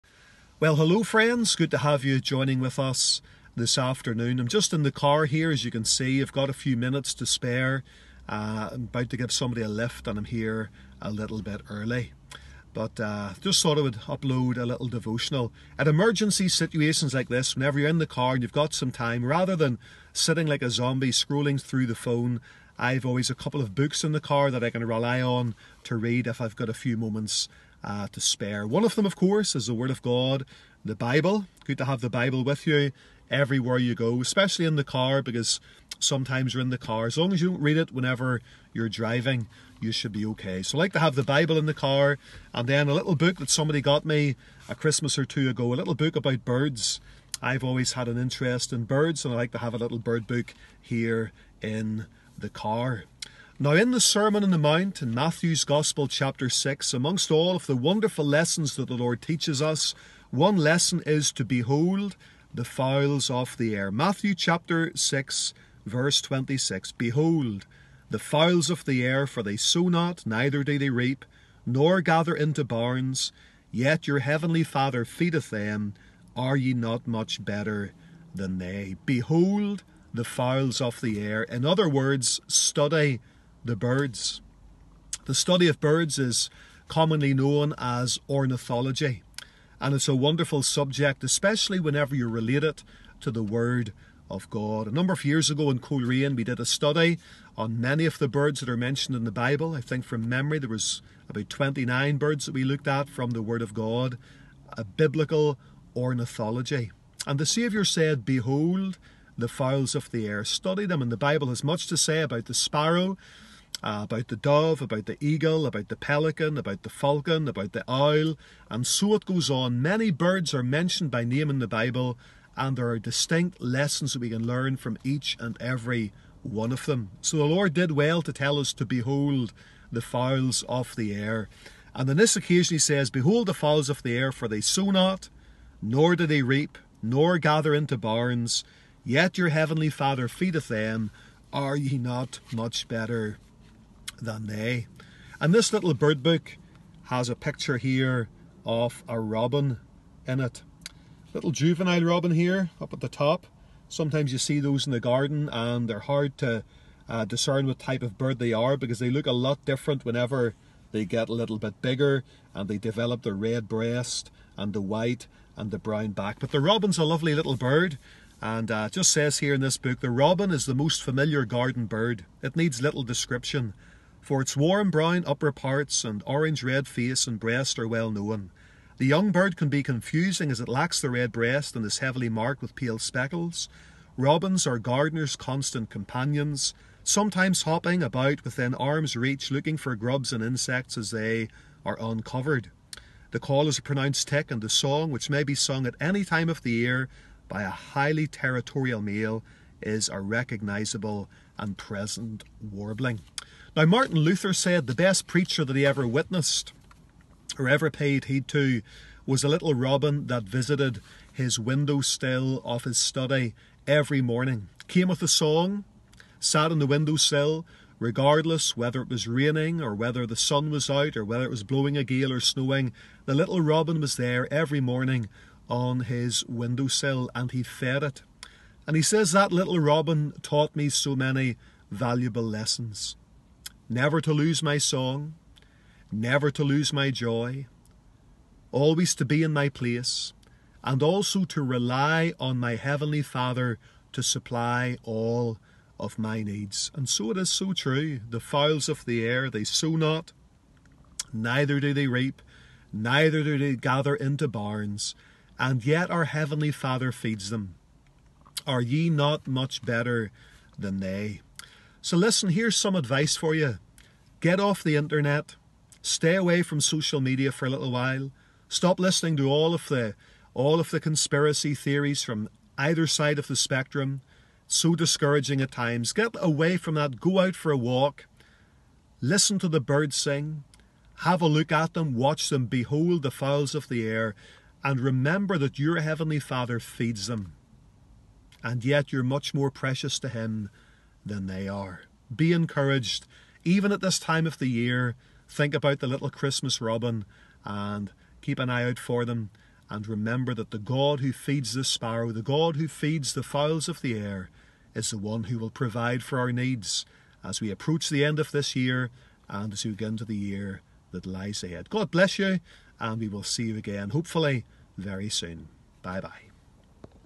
211223 Daily Devotional The Robin
Daily Devotional for Thursday 23rd December 2021.
"The_Robin" by Rev. Roger Higginson